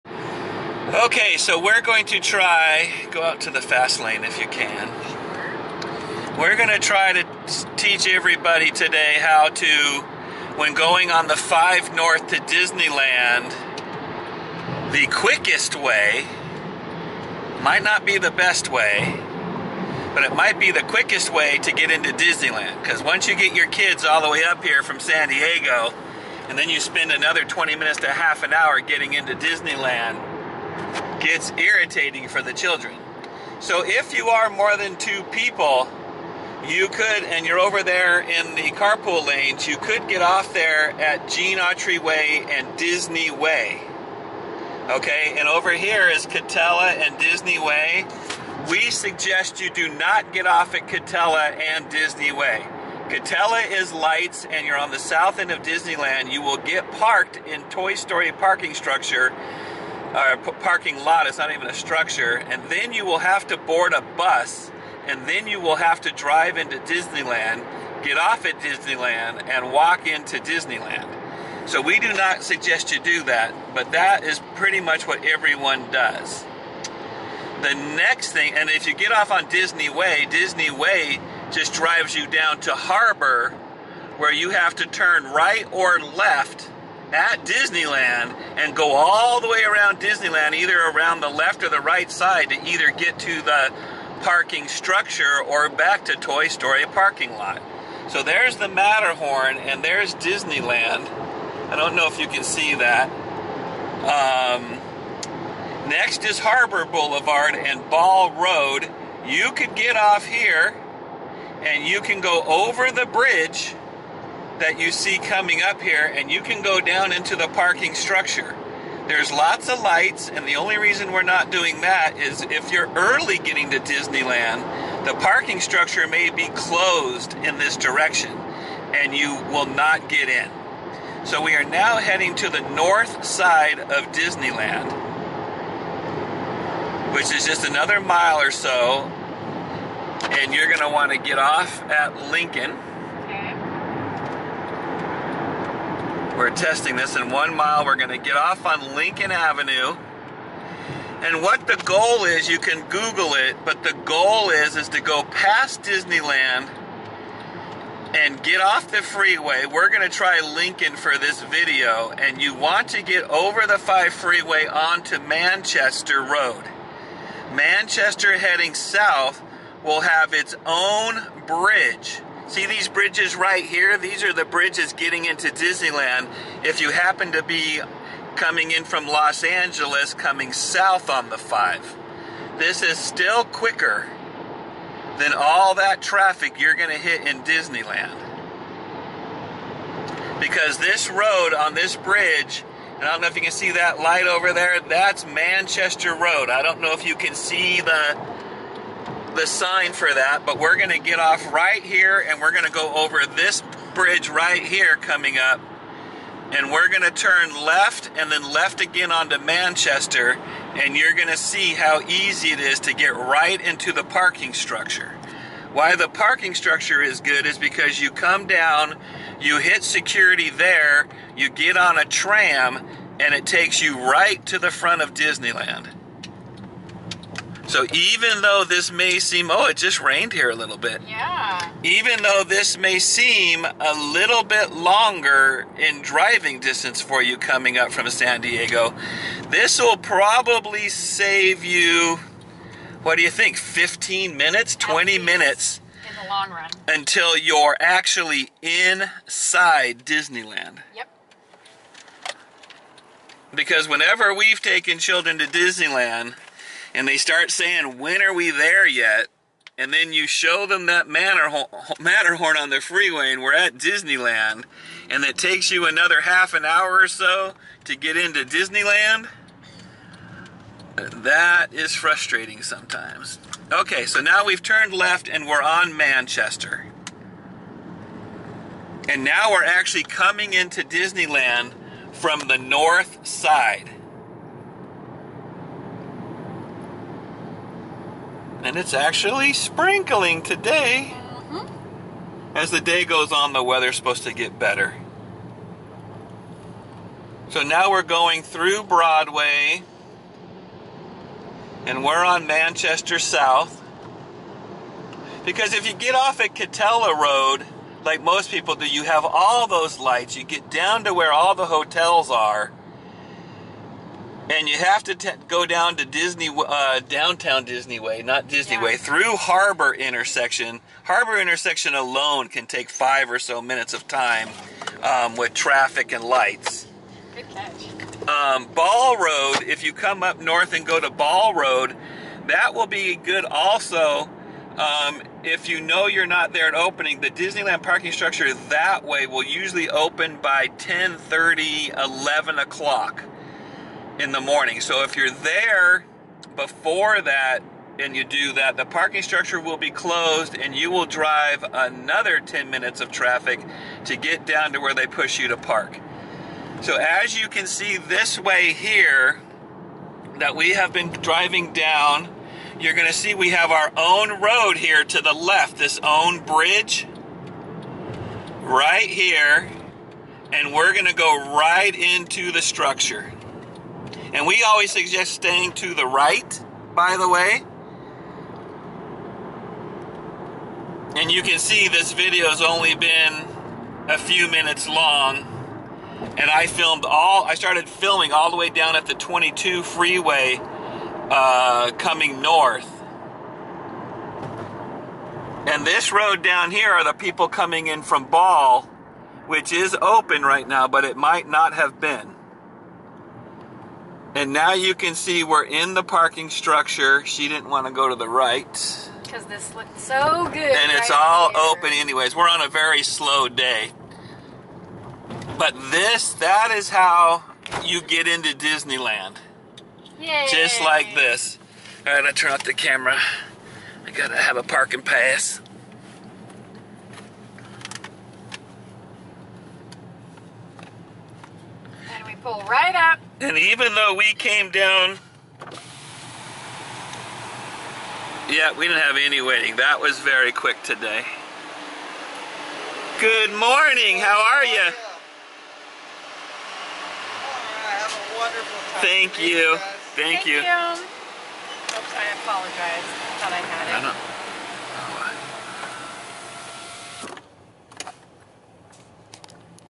The best way to park and drive into Disneyland like a pro and save a lot of time
Hi Everybody, In today's video we show you the best way to drive into and park at Disneyland. Secrets only us pros know. We start the video from the 5 freeway heading North just passing the 22 freeway and we are in the parking structure in just a few minutes. This is how to do it.